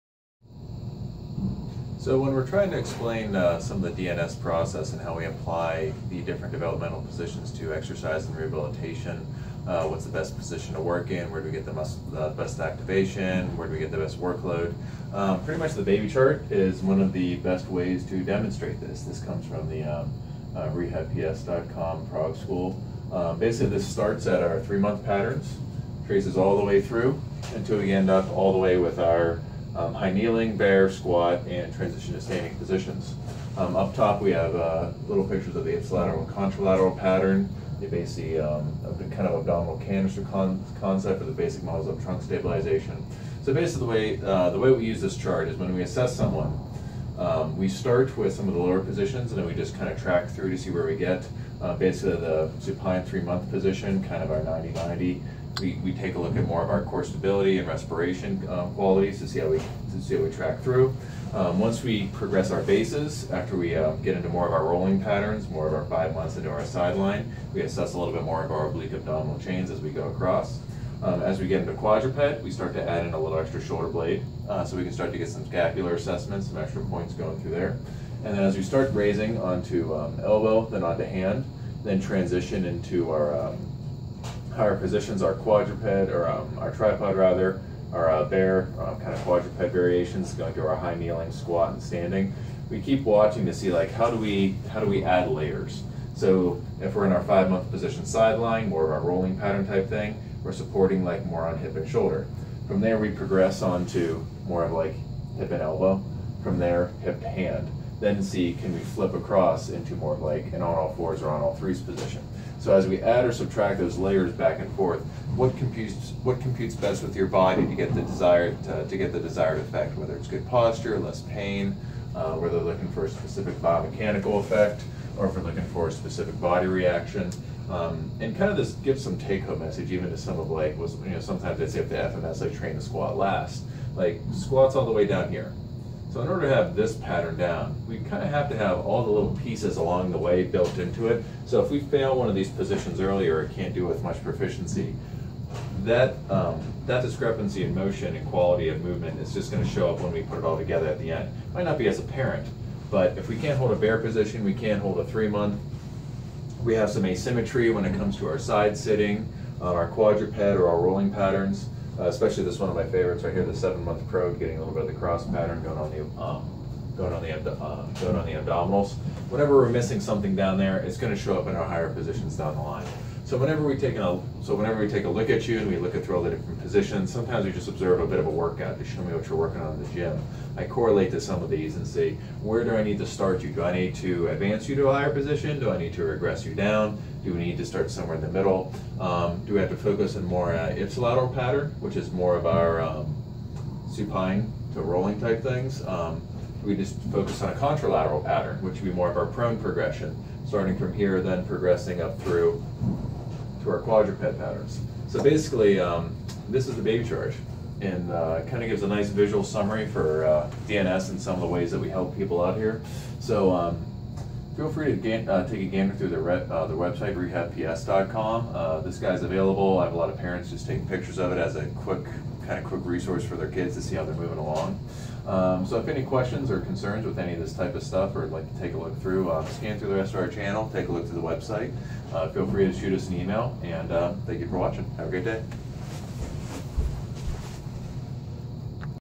DNS Baby Chart Overview - Applying Developmental Progression
I talk a bunch about applying the baby chart from dynamic neuromuscular stabilization, rehabps.com, to rehab and exercise, some of the ways we utilize the pathway to design our programs.
It has become a key part of my treatment after completing the DNS exercise certification along with Clinical parts A,B and 3 courses of Vojta Reflex Locomotion